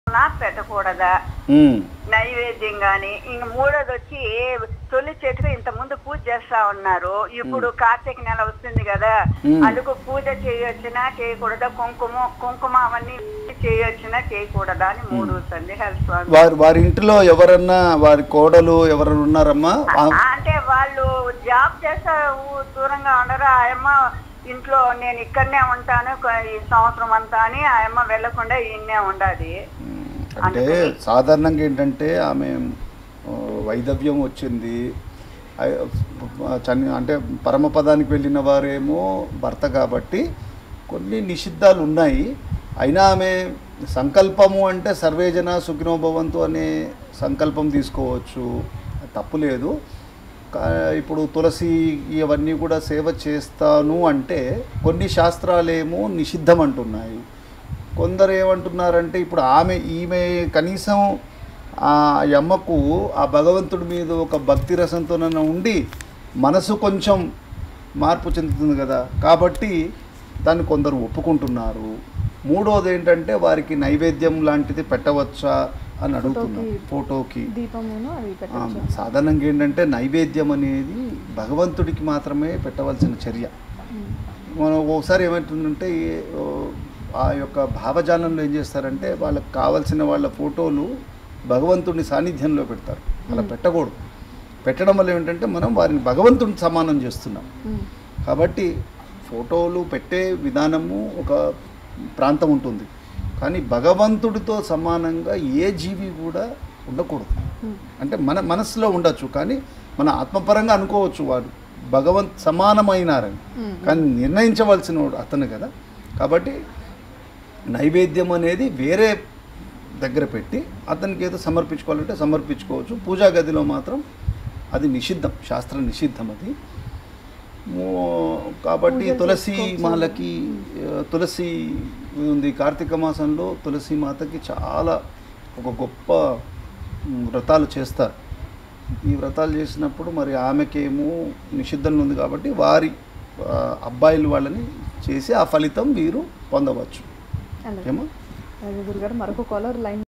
చనిపోయిన వారి ఫోటో దేవుని మందిరం లో పెట్టవచ్చా ? |CVR OM
సనాతన ధర్మం!# శ్రీమాన్ తోనేపర్తి కోదండ రామ శ్రీధర్ గారు
CVR OM Live | CVR Spiritual Channel | CVR OM